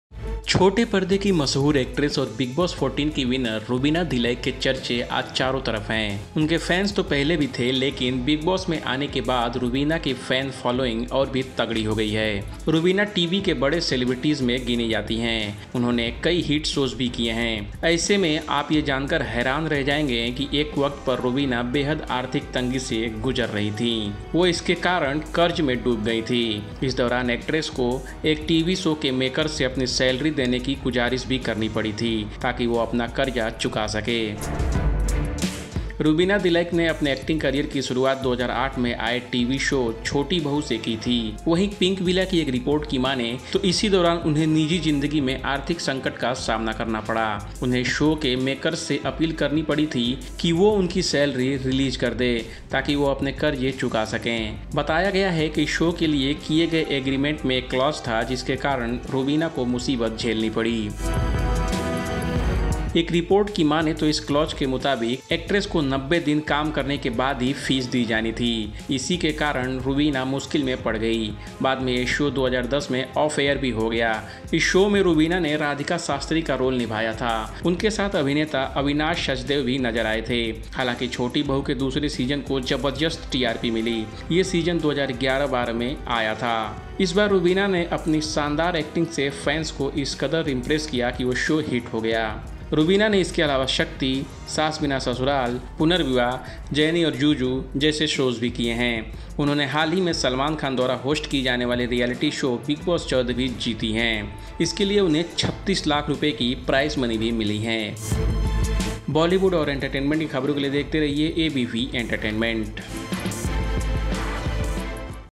When Rubina Dilaik's Financial Condition Had Deteriorated in Her First Show Chotti Bahu
Speaking about the non-payment of dues, Rubina told Pinkvilla in a 2019 interview, “I even played double roles in harsh conditions, yet I wasn’t paid my dues for the last three months, which were in lakhs. For nine months, I pleaded for my money, went to the actors association for help, but no one cared or helped apart from [making] big fancy promises. I had to sell off my house and property because I couldn’t pay off my EMIs. After crying my heart out, the production house finally called me up for negotiation."